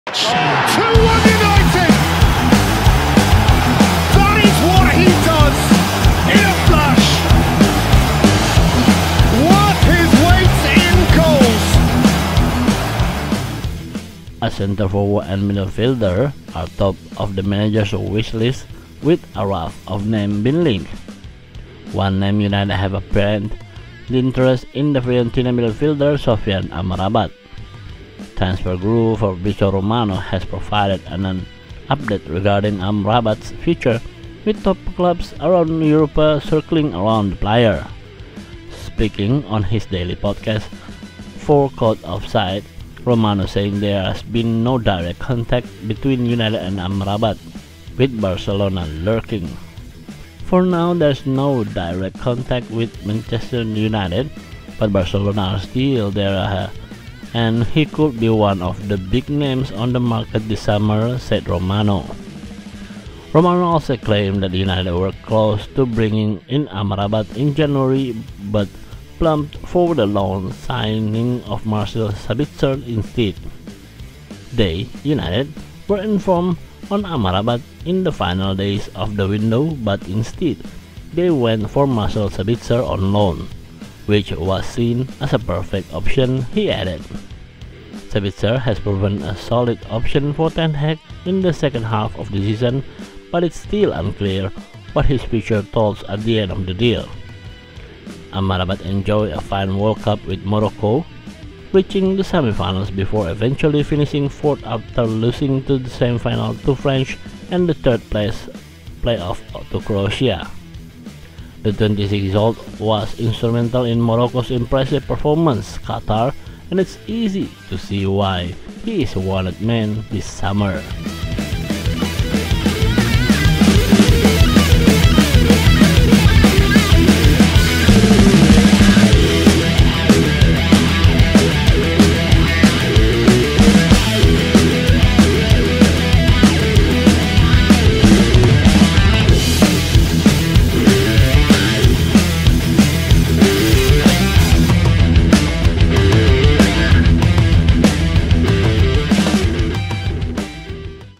Fabrizio Romano confirms United’s ongoing interest in Serie A midfield destroyer
A centre forward and midfielder are top of the manager’s wish list, with a raft of names being linked.
One name United have apparent interest in is Fiorentina midfielder, Sofyan Amrabat.

Transfer guru, Fabrizio Romano, has provided an update regarding Amrabat’s future, with top clubs around Europe circling around the player.
Speaking on his daily podcast for Caught Offside, Romano says there has been no direct contact between United and Amrabat, with Barcelona lurking.

“For now, there’s no direct contact with Manchester United, but Barcelona are still there and he could be one of the big names on the market this summer,” said Romano.
Romano also claimed that United were close to bringing in Amrabat in January but plumped for the loan signing of Marcel Sabitzer instead.

“They (United) were informed on Amrabat in the final days of the window but instead they went for Marcel Sabitzer on loan, which was seen as a perfect option,” he added.

Sabitzer has proven a solid option for Ten Hag in the second half of the season but it’s still unclear what his future holds at the end of the deal.

Amrabat enjoyed a fine World Cup with Morocco, reaching the semi finals before eventually finishing fourth after losing the semi to France and the third place playoff to Croatia.
The 26-year-old was instrumental in Morocco’s impressive performance in Qatar and it’s easy to see why he is a wanted man this summer.